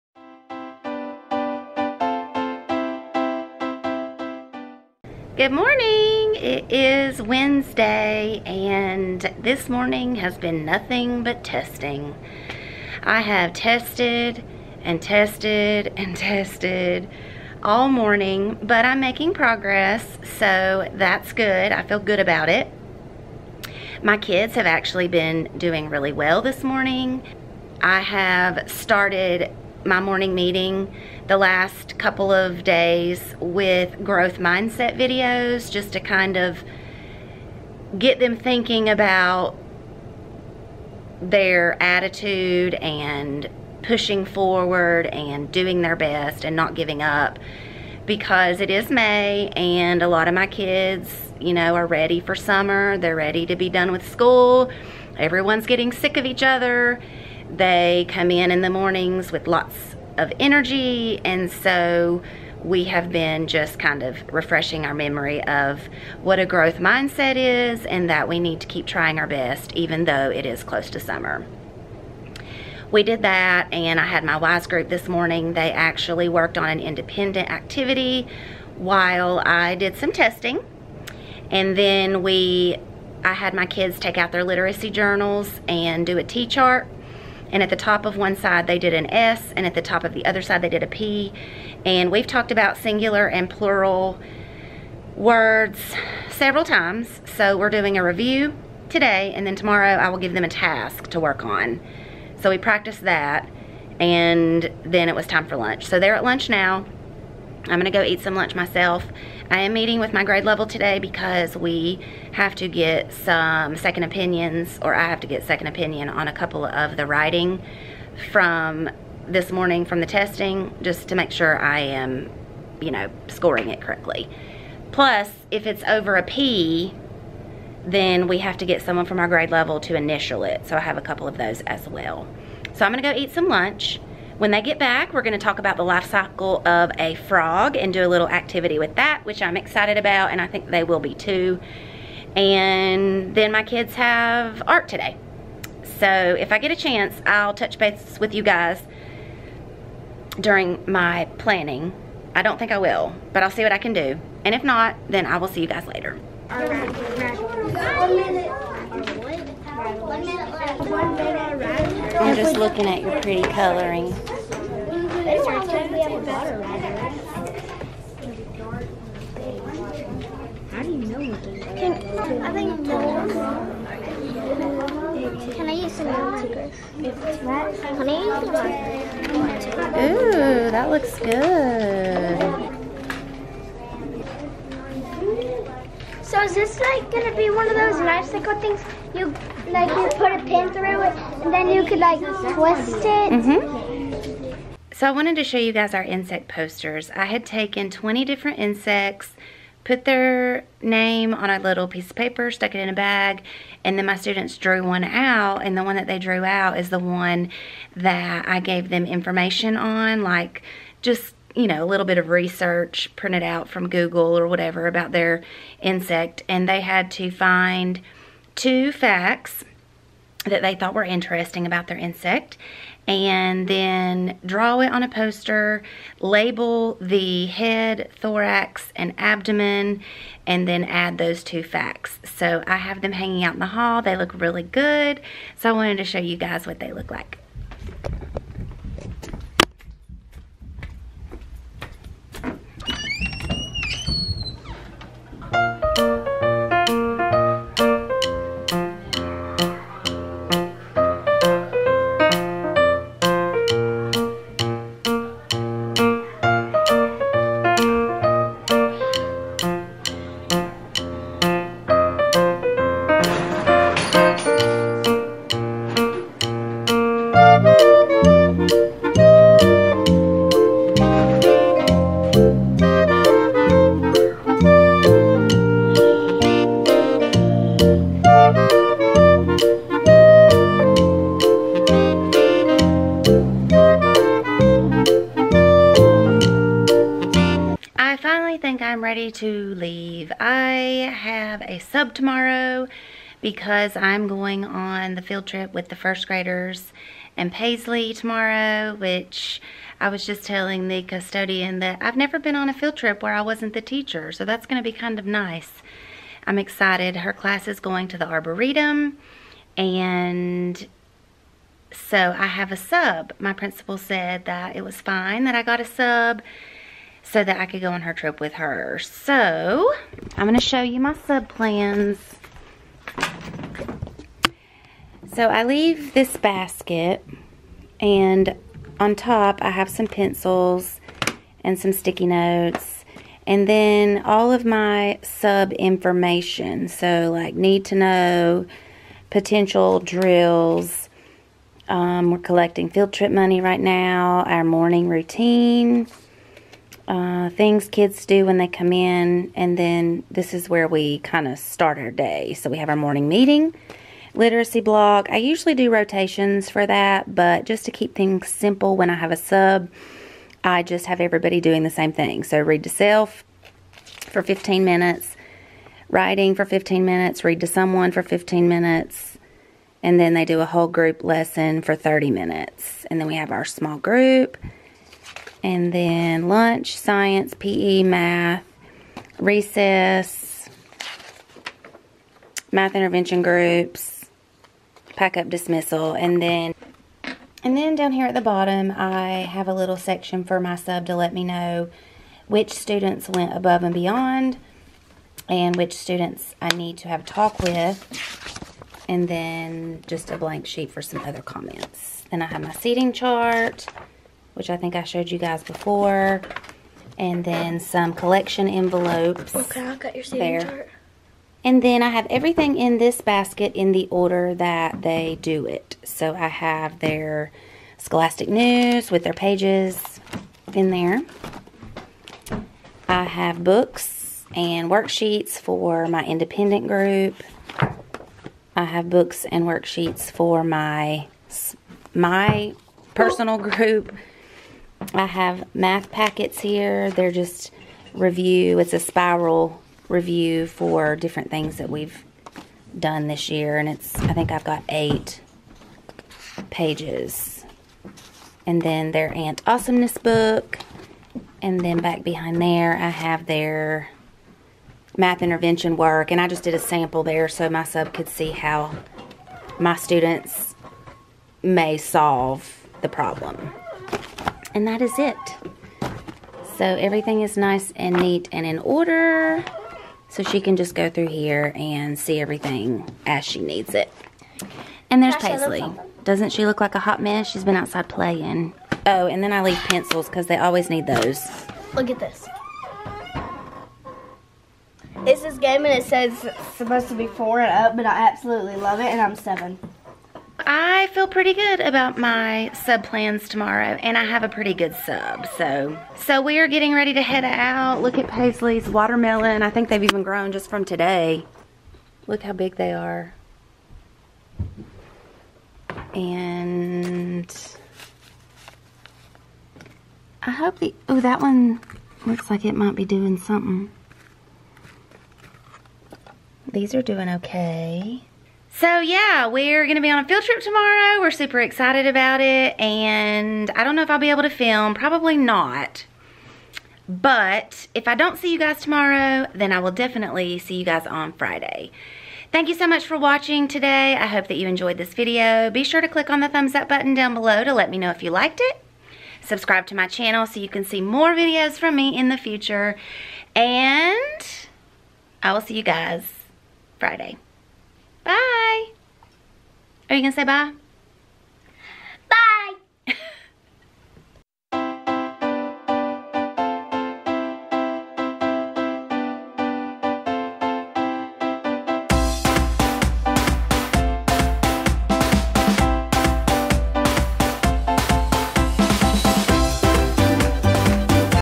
Teacher Vlog 2018 | That Teacher Life | 2nd Grade
Welcome To "That Teacher Life!" Join me and my 2nd grade class where I vlog our day, everyday! Teaching is my passion and I hope that you'll come on this journey with me!